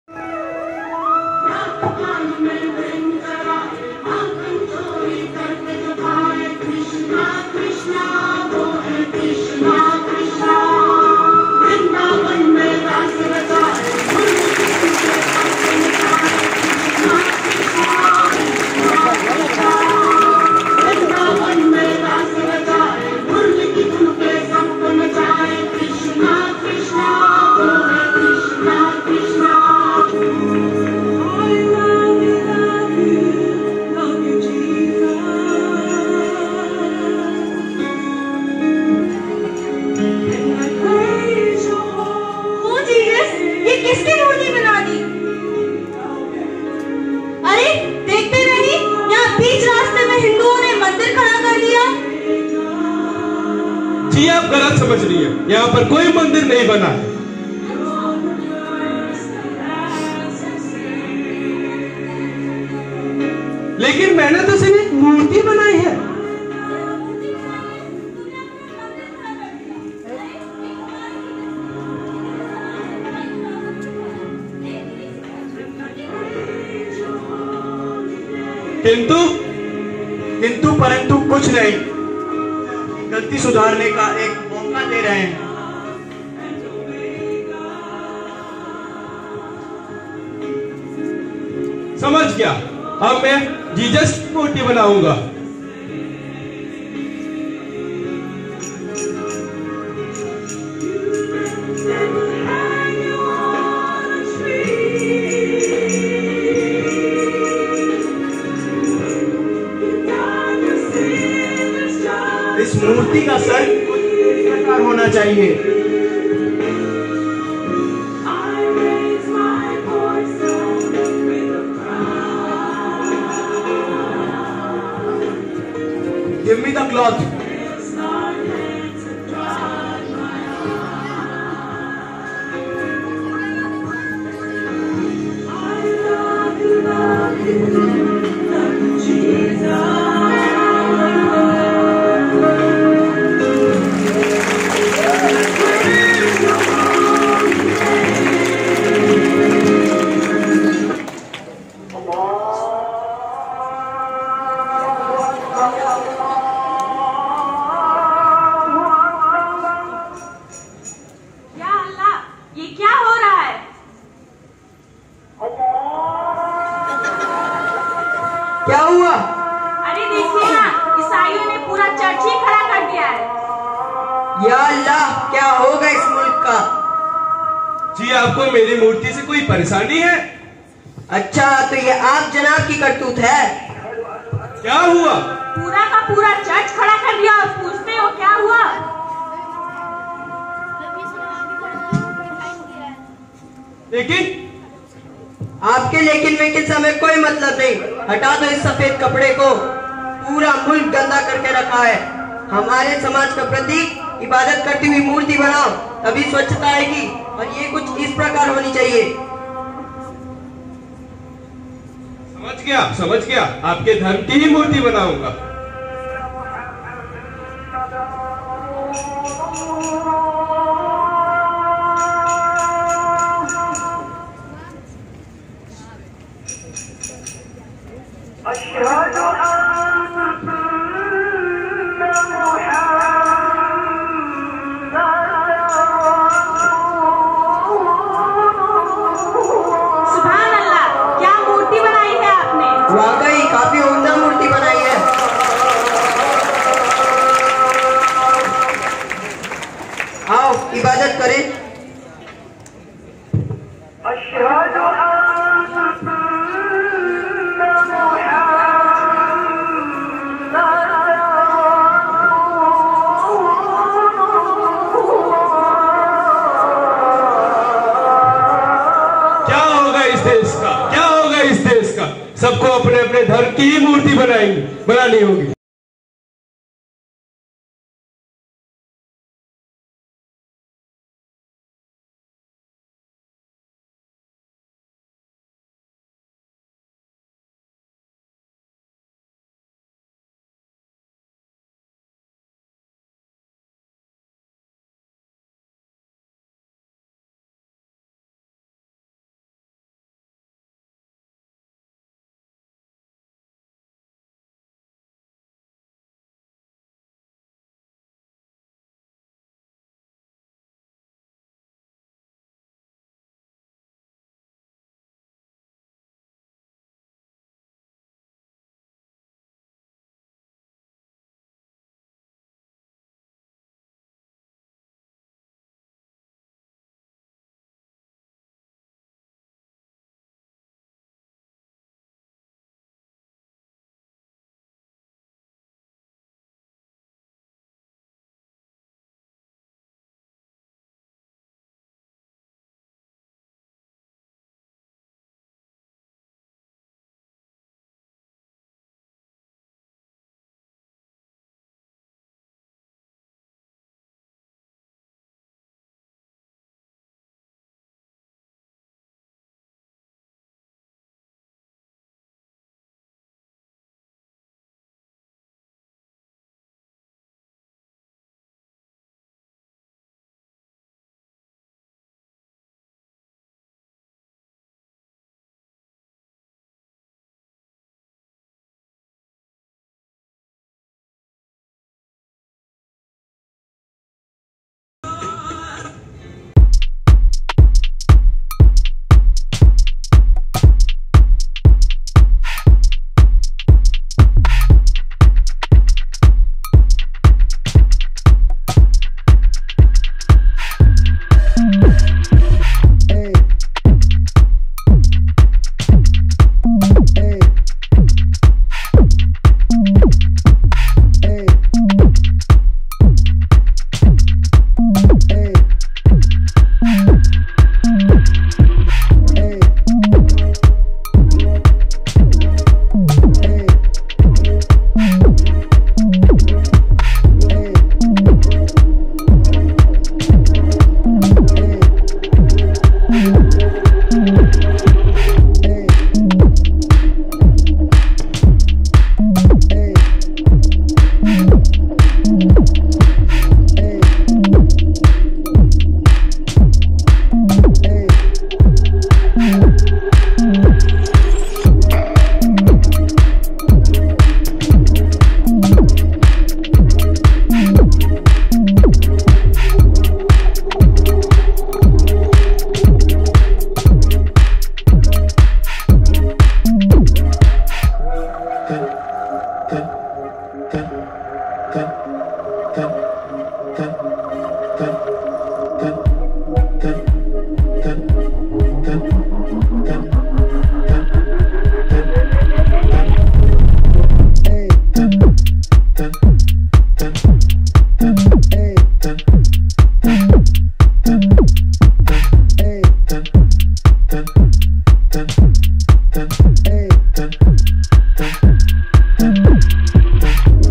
(Republic day)Drama act in APS kunraghat 2018-19 Presented by Sumit Kr. and music by: Mayank
If you like this comment and share ..